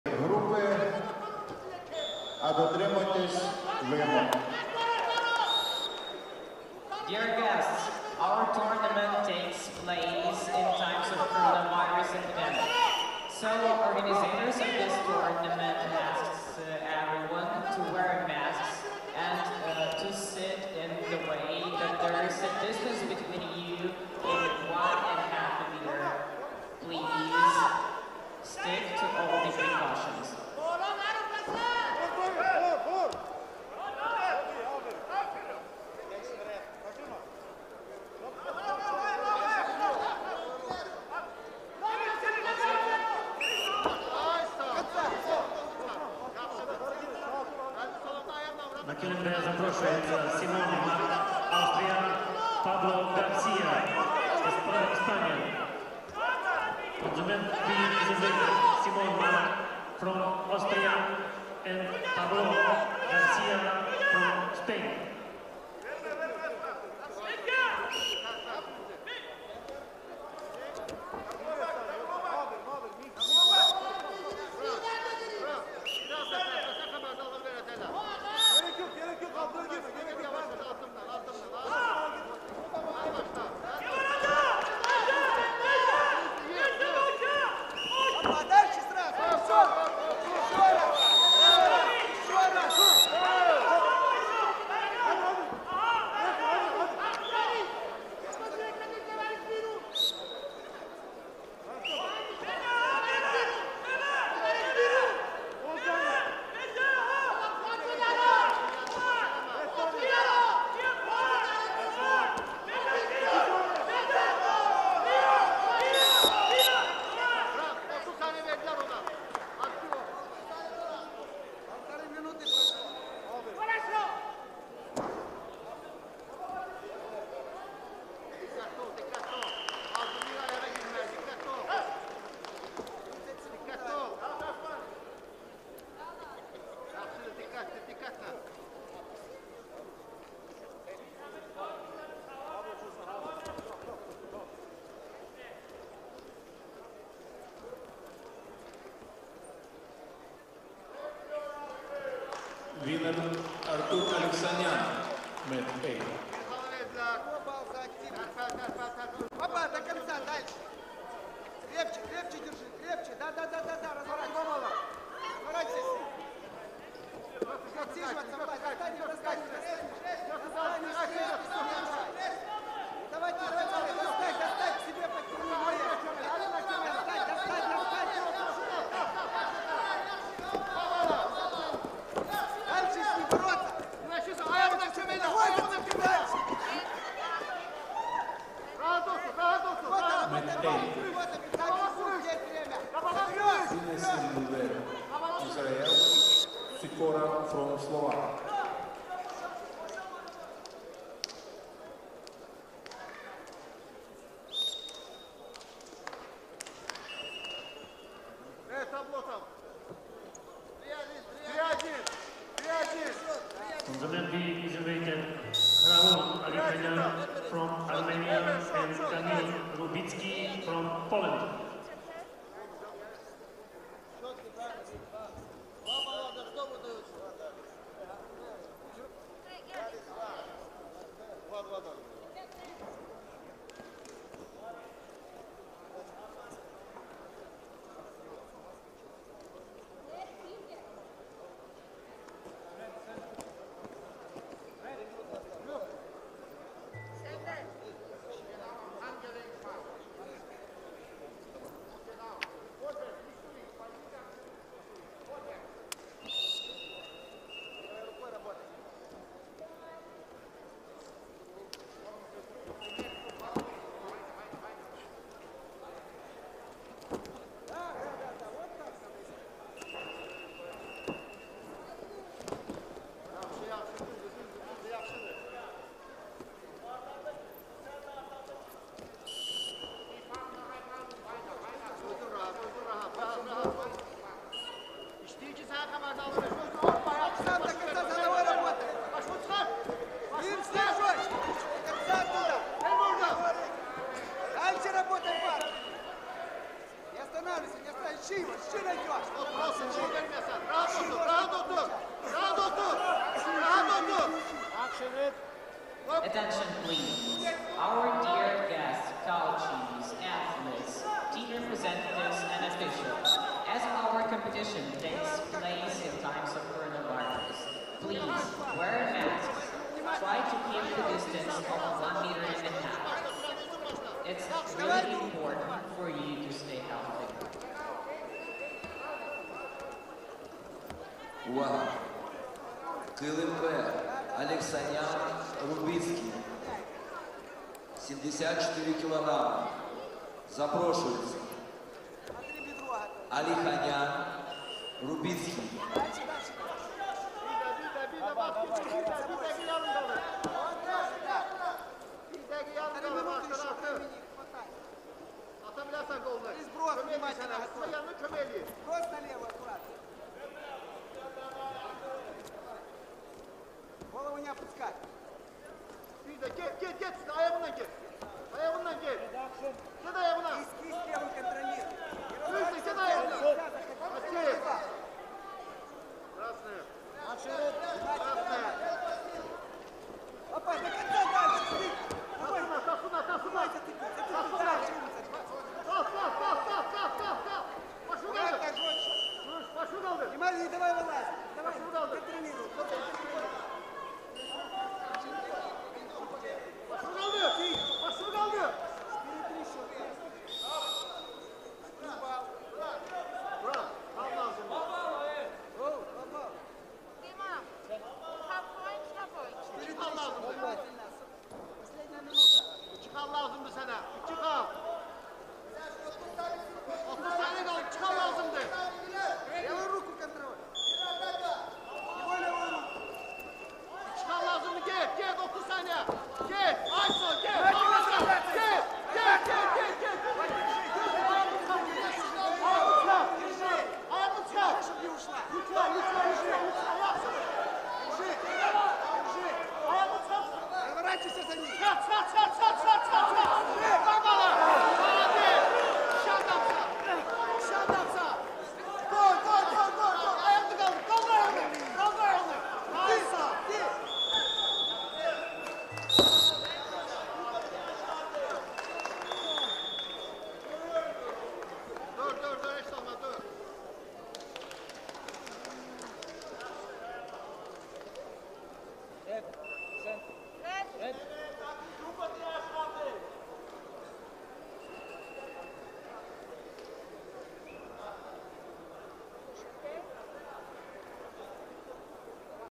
1/16 final FS - 65 kg: Qor Ohannesyan (Ukrayna) - Turan Bayramov
Kiyev turniri - 2021